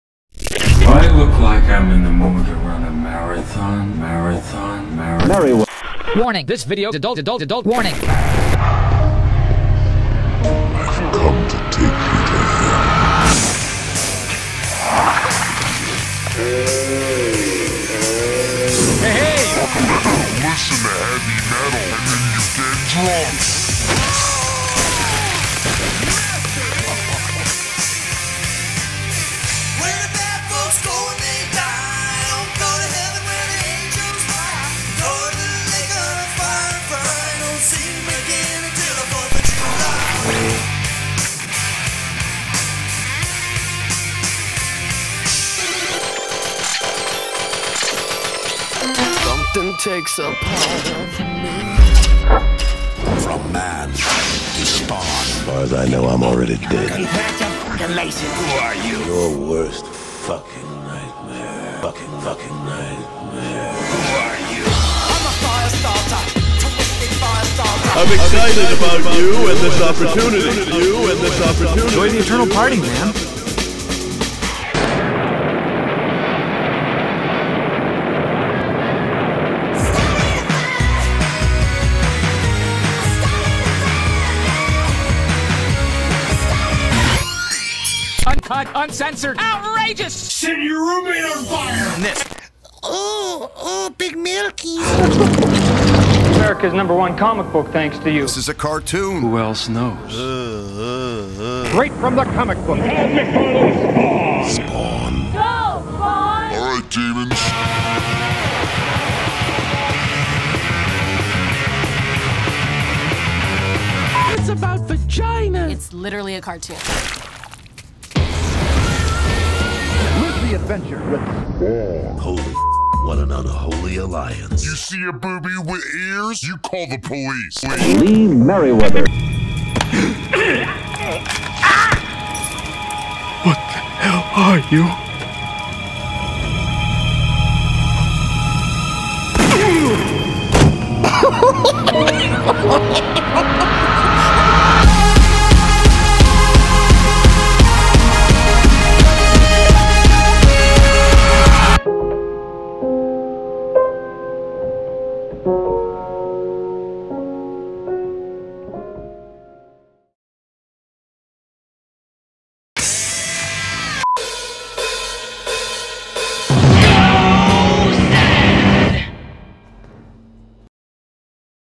Hell Comes To ToonTown!!! | Monster Fam
EVERY SUNDAY!! K!LLmation Presents!!!

Join The Monster Fam for an Animation Mini-Marathon in which we celebrate all things Unholy & Violent! Every week!!

Hail Satan & Watch Cartoons!!!